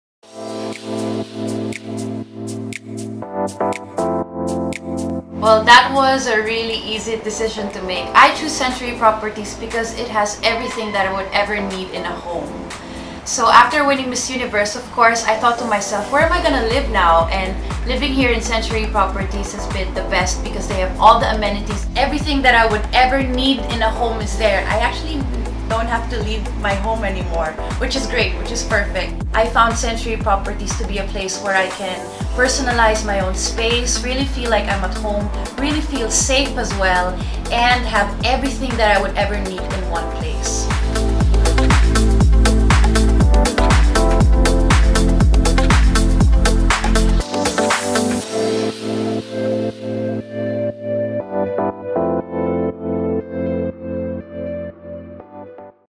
Century Stories: Pia Wurtzbach | Century Properties
After winning Miss Universe and living abroad for a year, Pia Wurtzbach thought about finding her own dream home. She found Century Properties to be the perfect choice as it has everything she would ever need in a home. “I found Century Properties to be a place where I can personalize my own space, really feel like I’m at home, really feel safe as well, and have everything that I would ever need in one place,” she shares. Watch the video and find out why she chose Century Properties.

Excited to see Pia’s condo unit? We are, too!

You, too, can OwnYourCenturyHomeToday! to learn more.